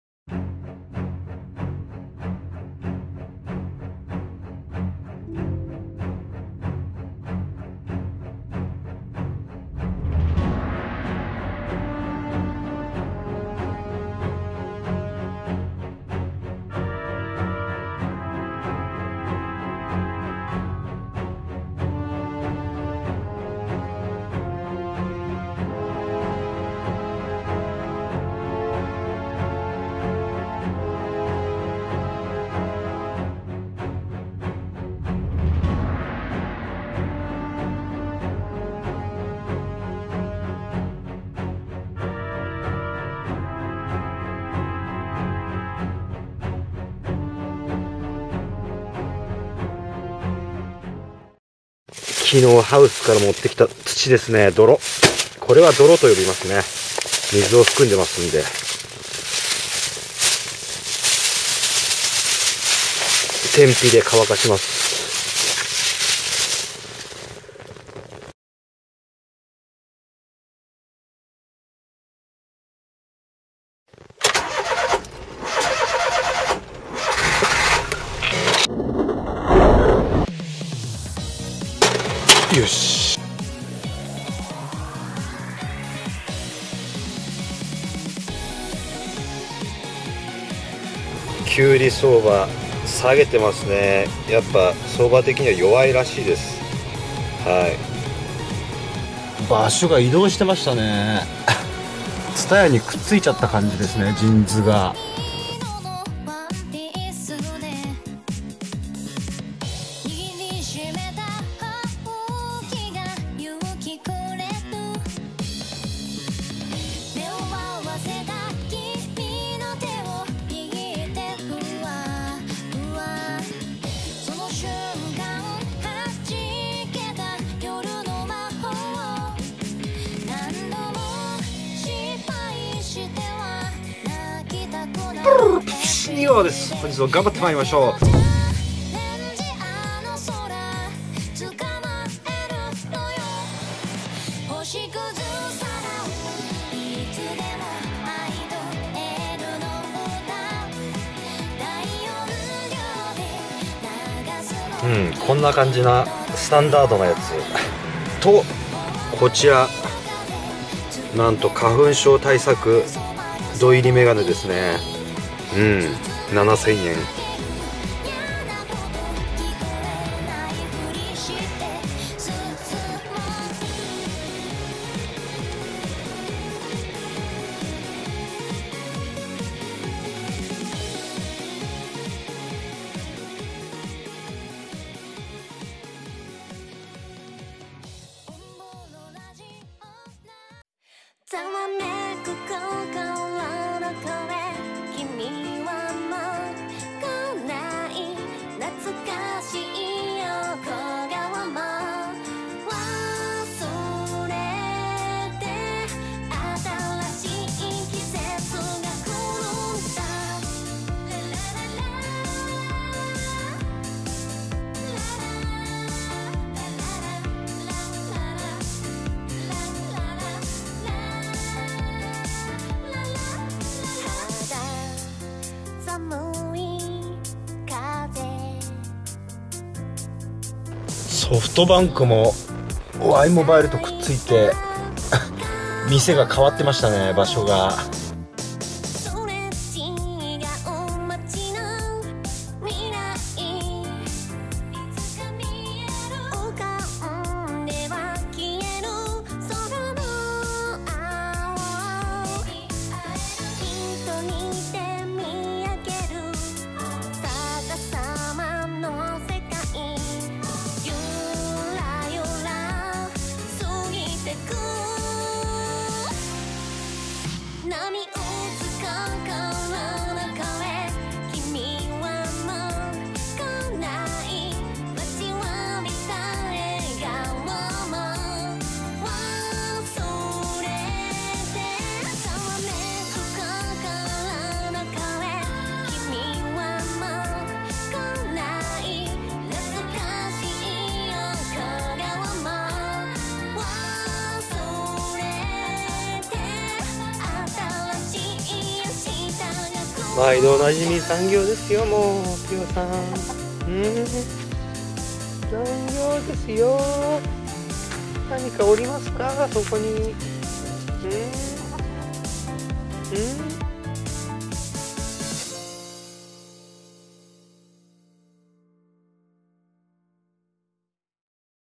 20190114 農業日誌 メガネ、スマホ新調しました!!本日はお疲れ様でした!!／きゅうり農家／きゅうり栽培／愉快なshata農園
ども！シャタ農園のシャタです！
どうやらスマホの更新時期に合わせて２年間隔の恒例行事に
なりそうです。実際、農業でスマホ、メガネは２年が限界。
私の場合ですが。